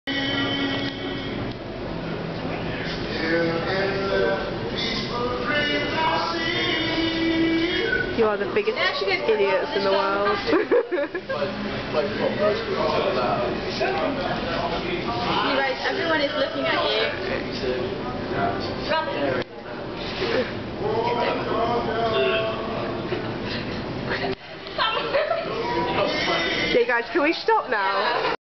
Dancing At Hut
Just A Little Gem I Found...It Was August '09...And Najerys 17th Birthday...Nothing More To Say other Than These Are Some Great Moves!. Or Maybe Not!
x